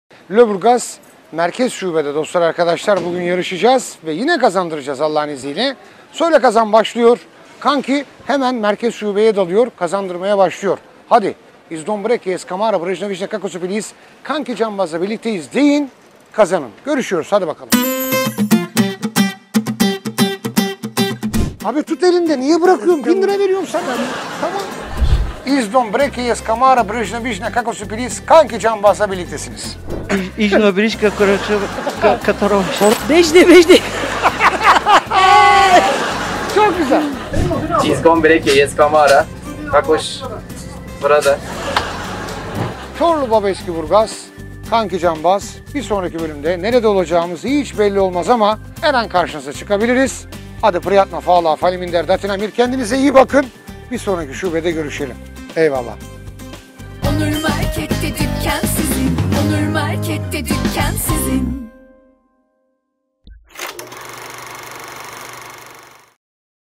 Söyle Kazan Lüleburgaz DFS Güncel Kanki Onur Market
Onur Markette Dükkan Sizin
Kral FM de Şarkılar Sizin
Anlatılmaz Lezzetlerde Mutfak Sizin
Söyle Kazanda Mikrofon Sizin
Söylersen 1000TL Hediye Çekide Sizin...

Söyleyen kazanıyor !
Yıllardır bizim kanki radyoda söyledi şimdi sıra sizde.
Onur Market’ten 1000 TL değerinde Onur Alışveriş Kartını kazanın.
Yarışan her Onur Market müşterisine 50 TL değerinde
Onur Alışveriş Kartı garanti.
Sende Söyle Kazan.
Her an bir Onur Market şubesinde
karşınıza çıkabilir bizim Kanki Canbaz.
Onur Market Bursa Kükürtlü Şubesindeyiz.

Abe Yes kANKİ cANBAZ...